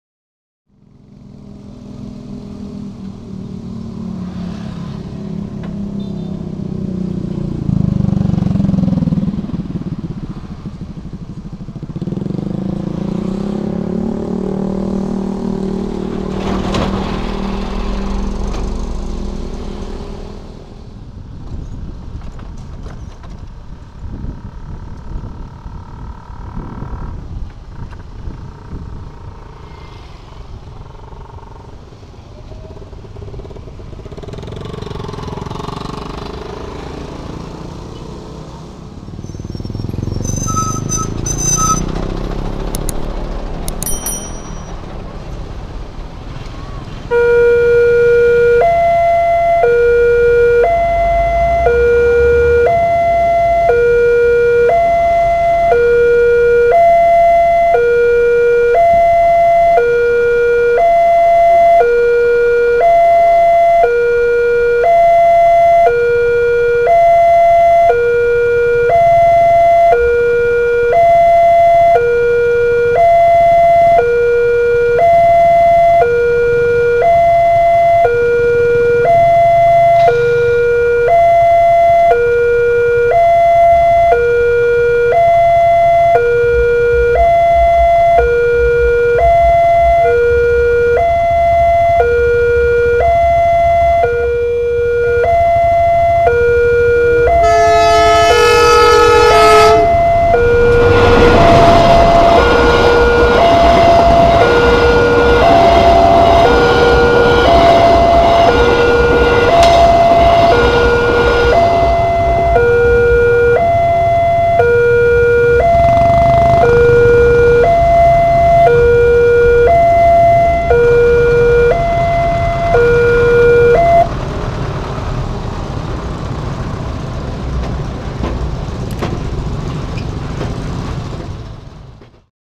PALANG PINTU PERLINTASAN KERETA API JPL 37 Tanpa Lampu Flasher Balapulang | Railroad Crossing
Palang Pintu Perlintasan Kereta Api JPL 37 tanpa Lampu Flasher di daerah Balapulang Kab. Tegal melintasi Kereta Api Kamandaka